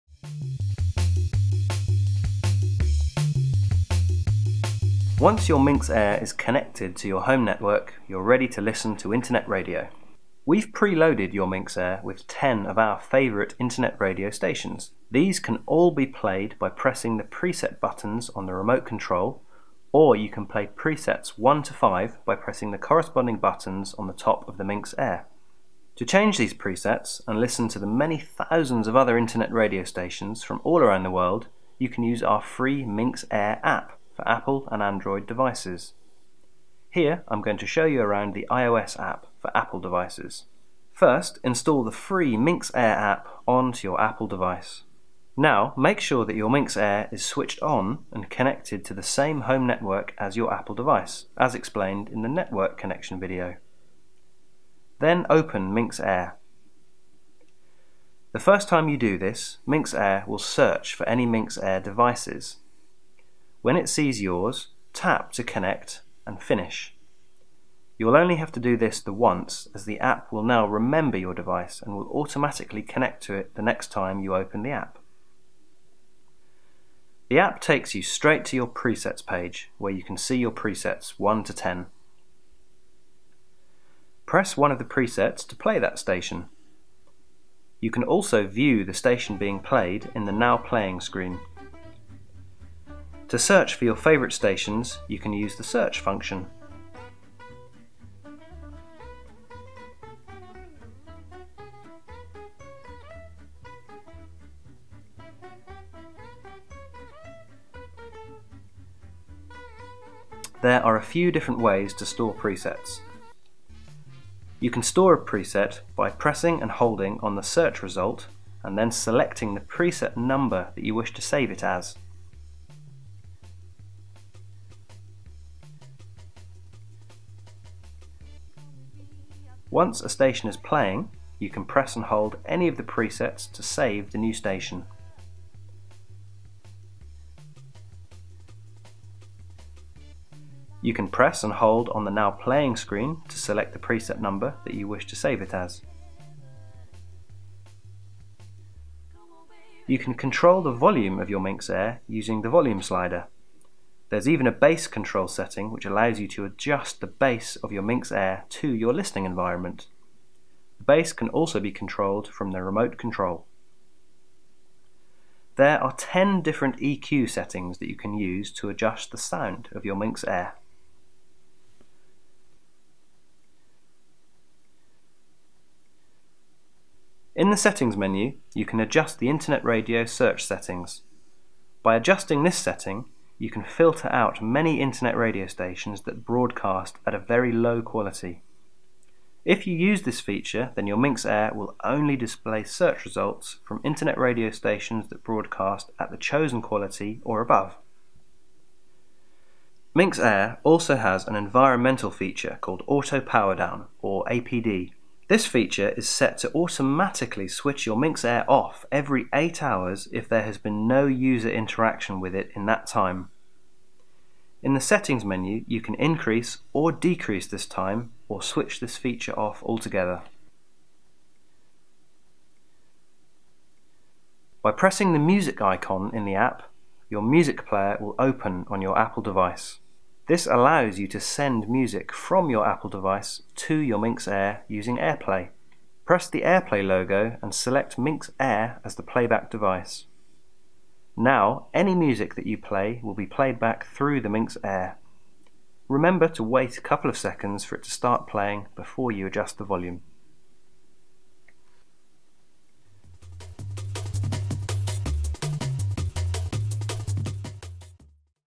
Minx Air - Internet Radio and iOS app
Minx Air is a revolutionary wireless system that plays all your music, wherever it is -- smartphone, tablet or on your computer.

Minx Air combines Cambridge Audio's expertise in hi-fi design with the latest wireless streaming technology. That means your smartphone can stay in your pocket, or you can keep using your tablet while your favourite songs fill the room.

So whatever device you're using, you can enjoy your music wirelessly in amazing quality.